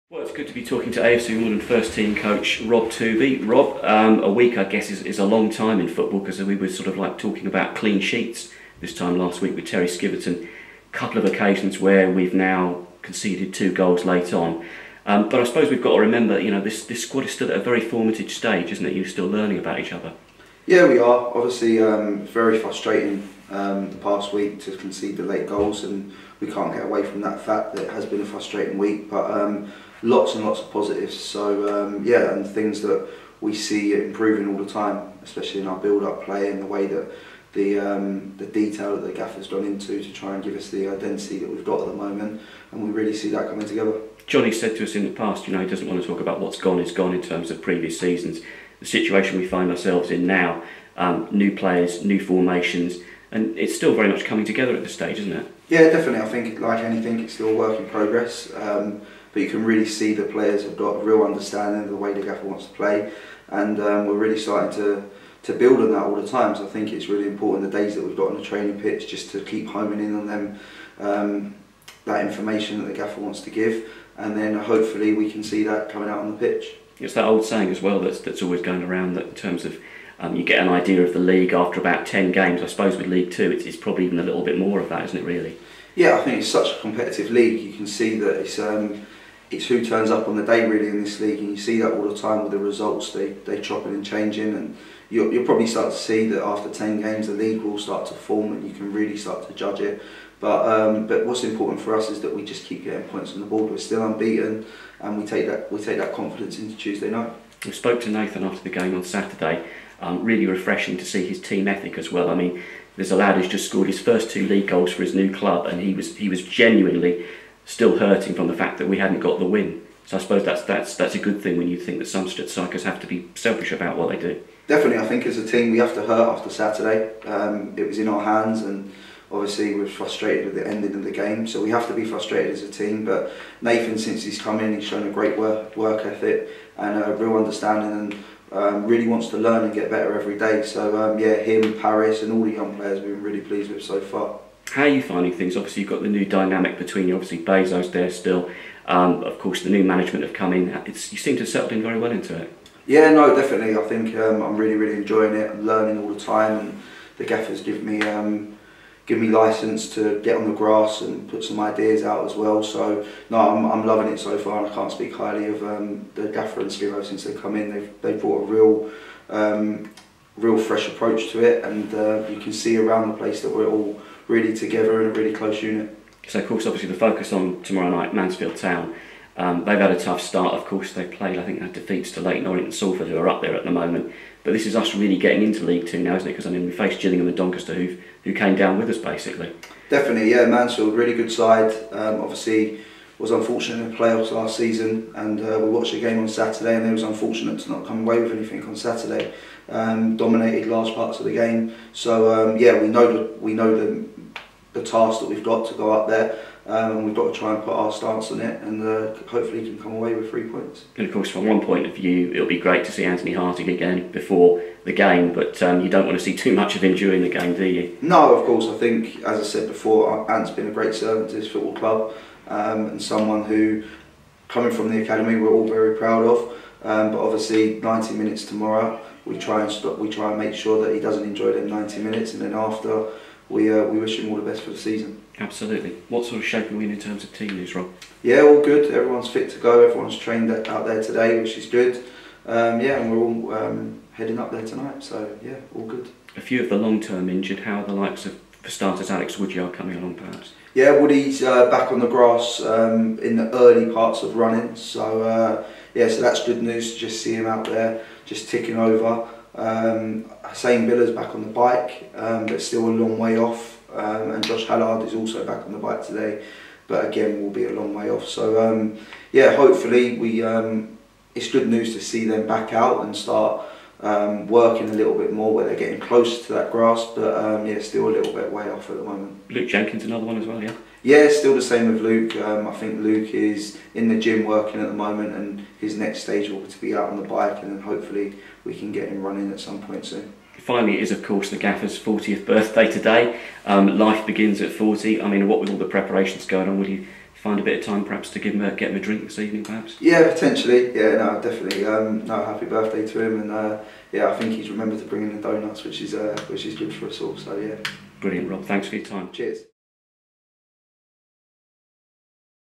💬 Rob's views on building an identity | Team news for midweek trip 🟡🔵
First team coach Rob Tuvey gave us his views after training on Monday and before setting off for Mansfield Town.